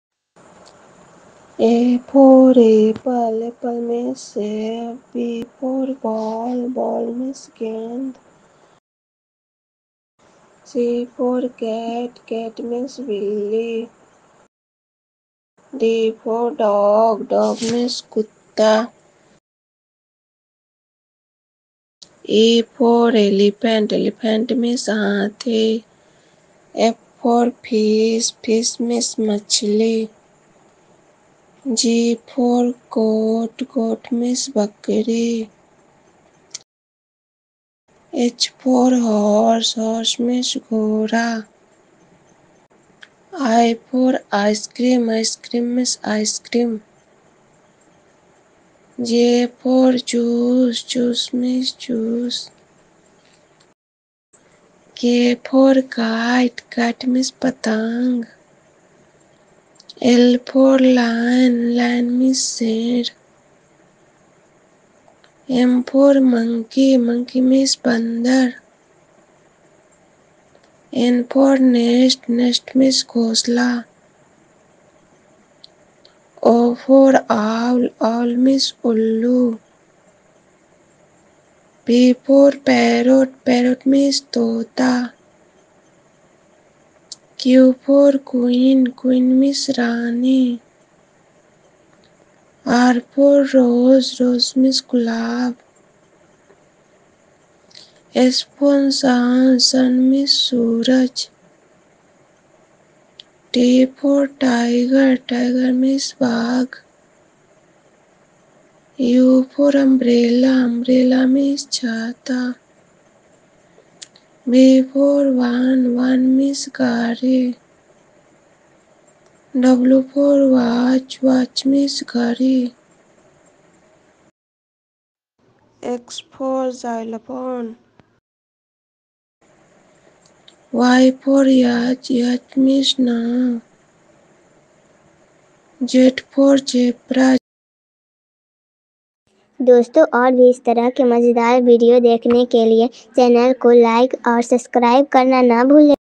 A B C D Learning | A for Apple, B for Ball with Example Image | Animated Graphic | English Learning
A B C D Kids Learning Video with Animated Graphics. This will help the child in learning a to z alphabets and words.

This will engage you in learning the alphabet in an interesting way and will also help to remember the letters in the alphabet.